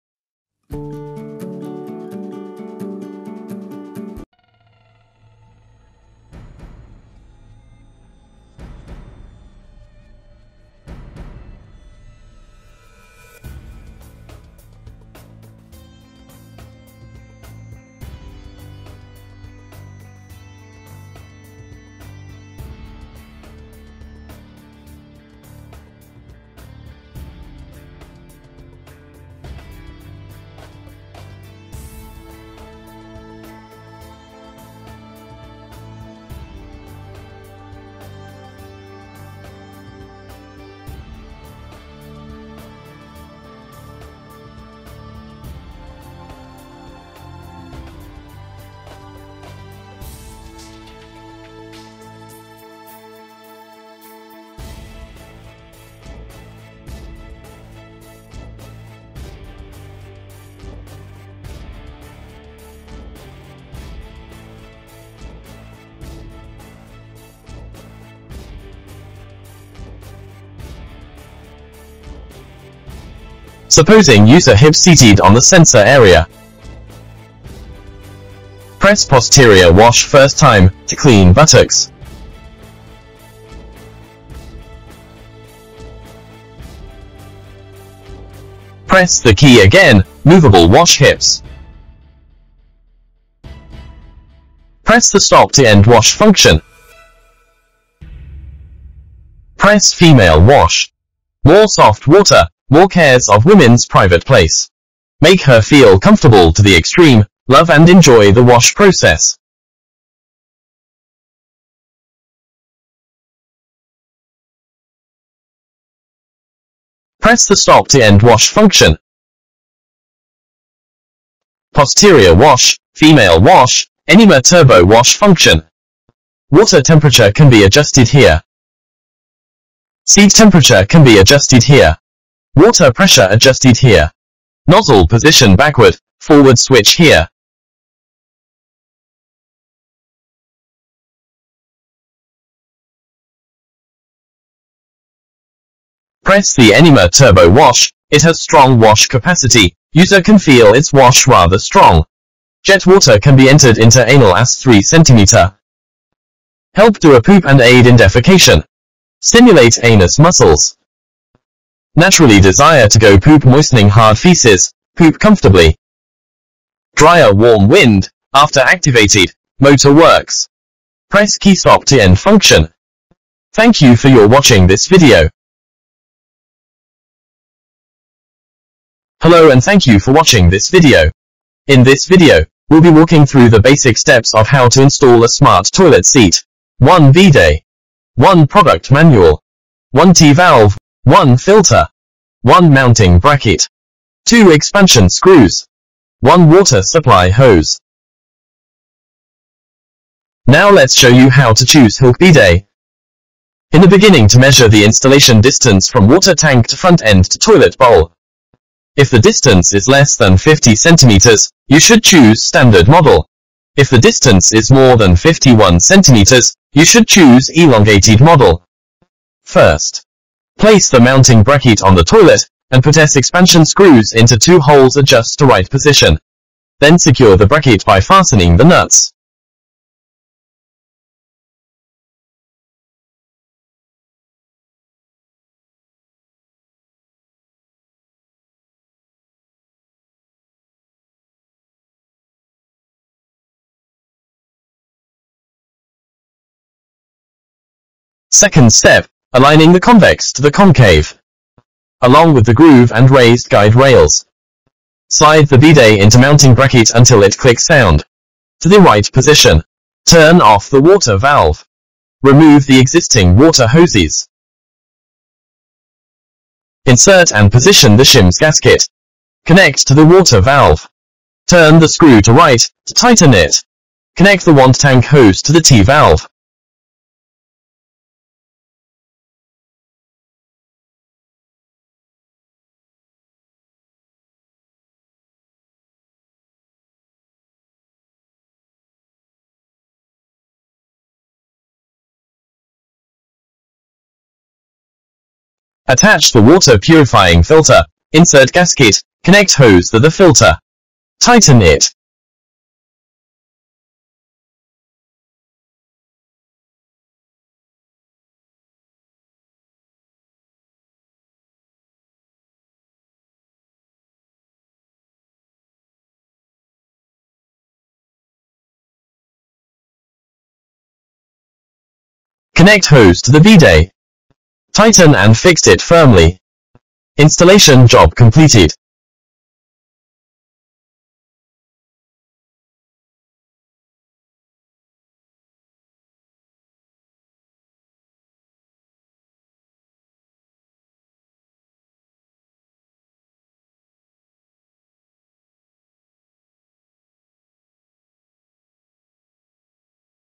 American style 110V -120V 60 HZ KB1500 Elongated Smart commode Intelligent Toilet bidet seat
High matching suitability: can match 90% toilet in the market. Have over 20 models bidet with different size and shape to applies to your toilet bowl.

Zhejiang Hilk Smart Home Co.,Ltd.
Largest OEM factory on Intelligent Smart Toilet